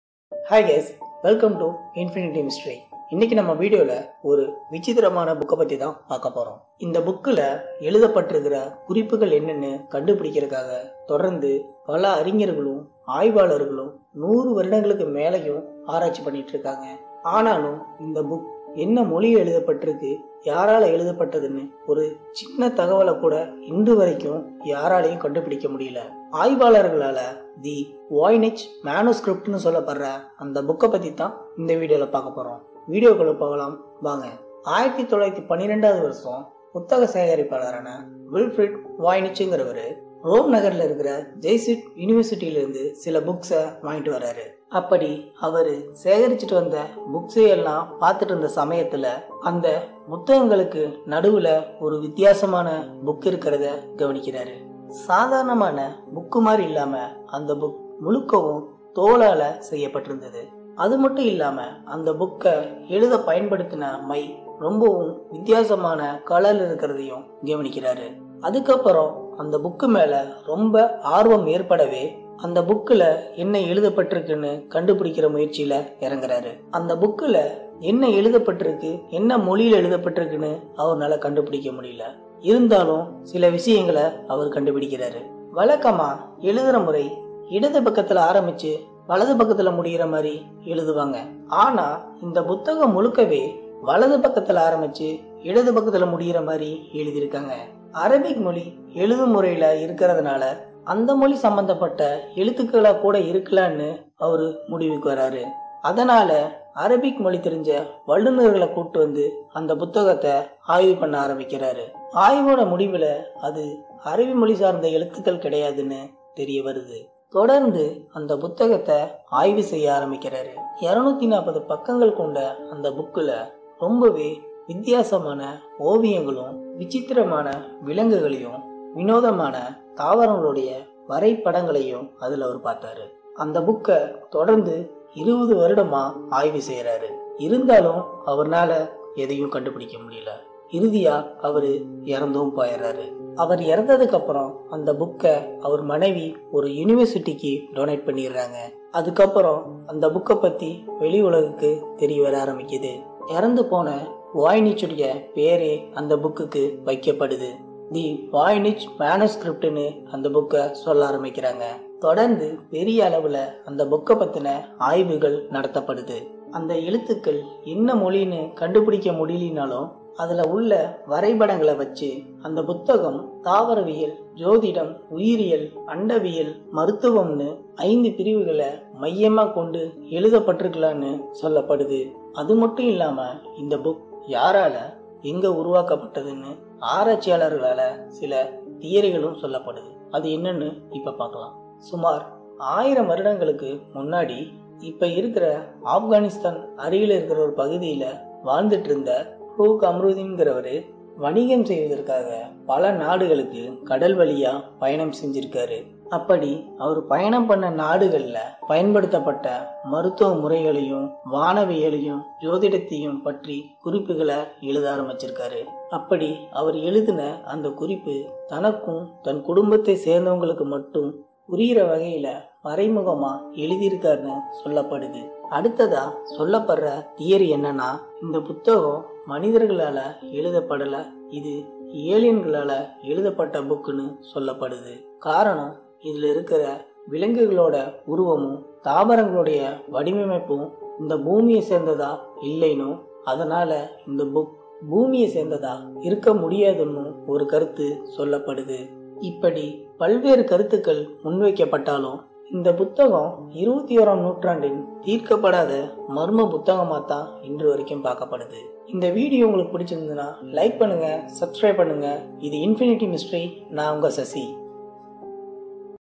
மனிதர்களால் படிக்க முடியாத மர்மா புத்தகம் | the world mysterious book of voynich manuscript.
This video about in world mysterys book of the voynich manuscript details in tamil.

Background free Music credits By,